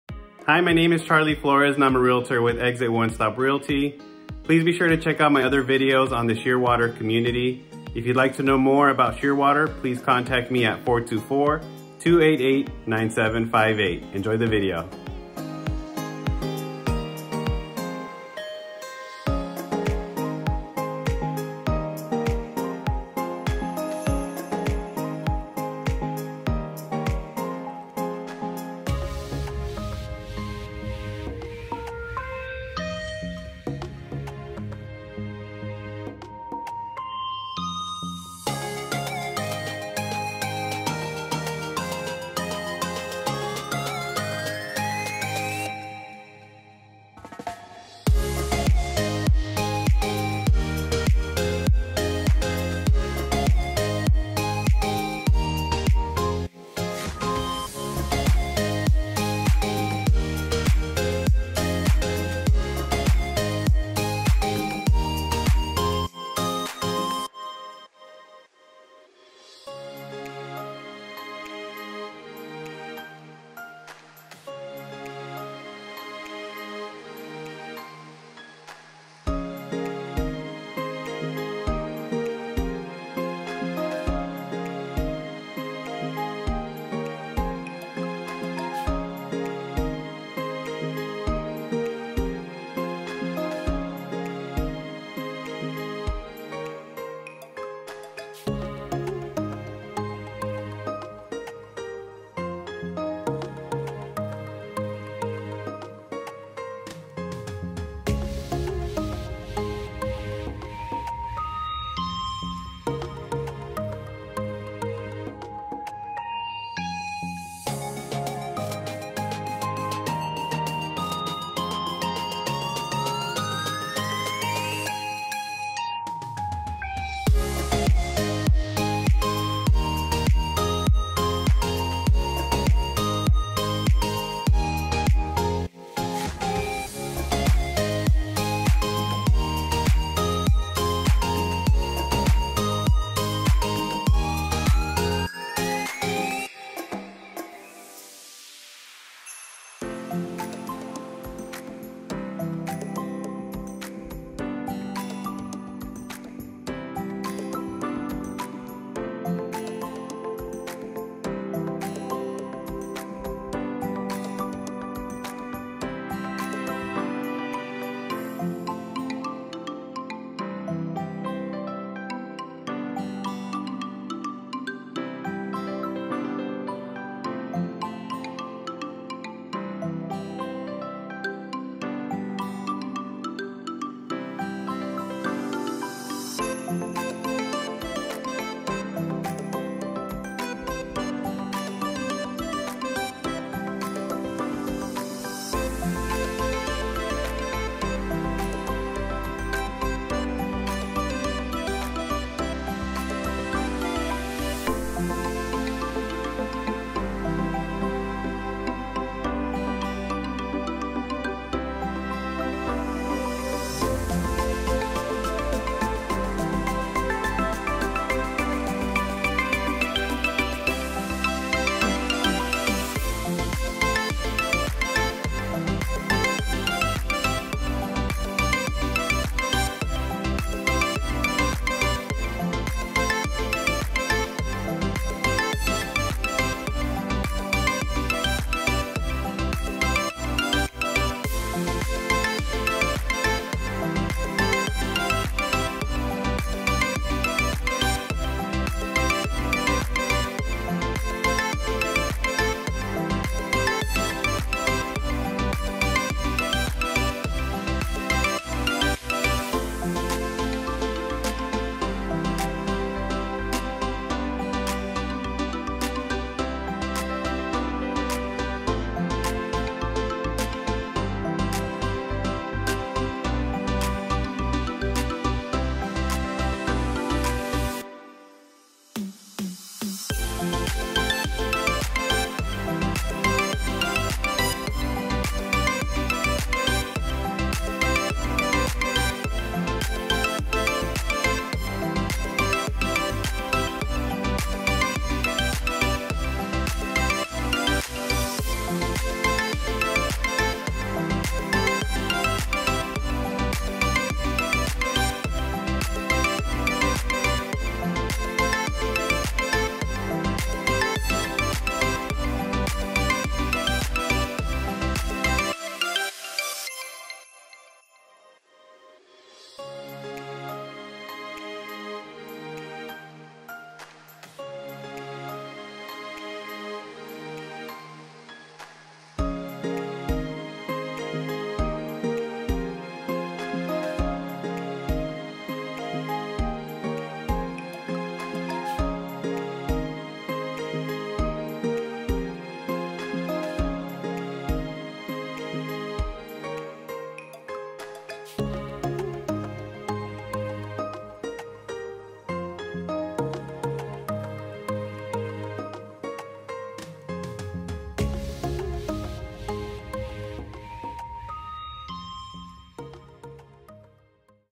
Montauk II by Dream Finders Homes at Shearwater
Featured in this video is the Montauk II model by Dream Finders Homes at Shearwater in St Augustine (in St Johns County), FL.
This floor plan has a total living area of approximately 3,688 squared feet and includes 4 bedrooms, 4.5 bathrooms and a 3 car garage.

For new construction homes, I do not work for the builders and have no control over their prices and incentives.
Model homes will have upgrades that will increase the price of that floor plan above the base price.